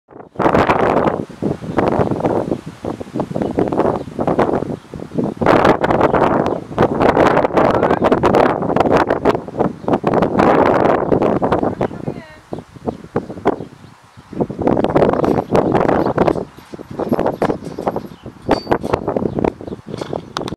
Beautiful Day Outside
When weather allows, riders at Pikes Peak Therapeutic Riding Center enjoy time outdoors in the warm Colorado sun during lessons.